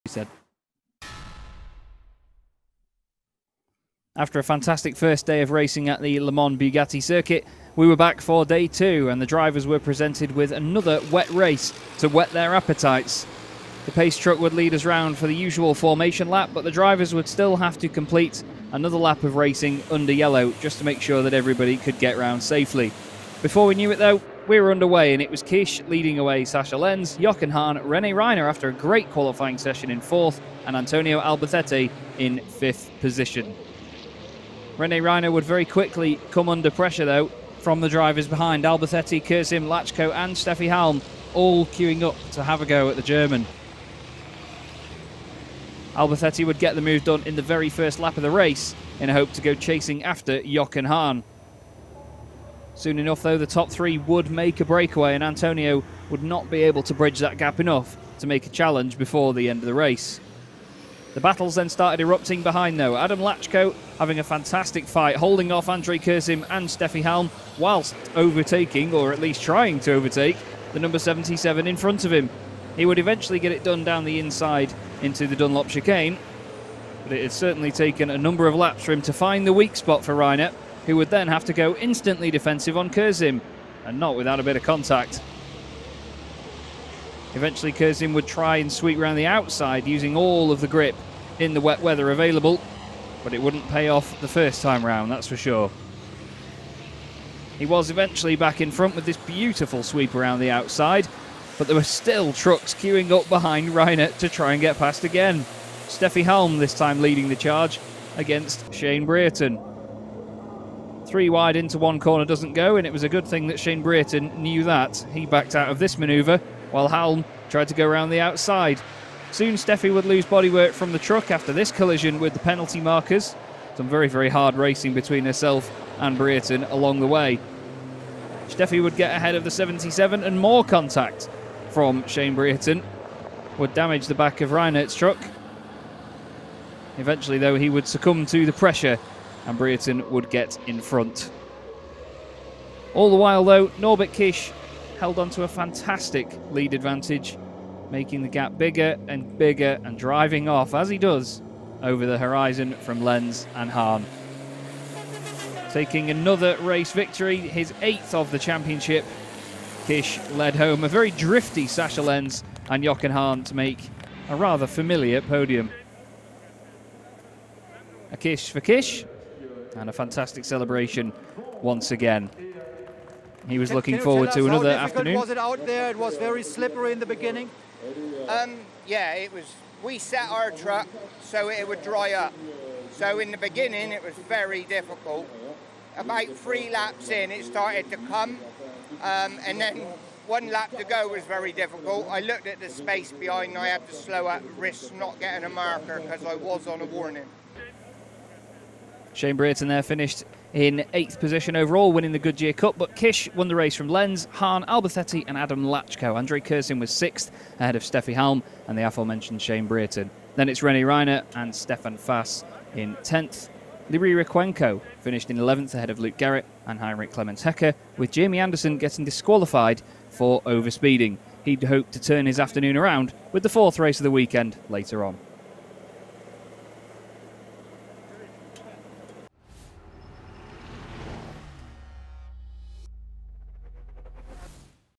2021 FIA ETRC Le Mans Race 3 highlights
Watch the highlights from race 3 at Le Mans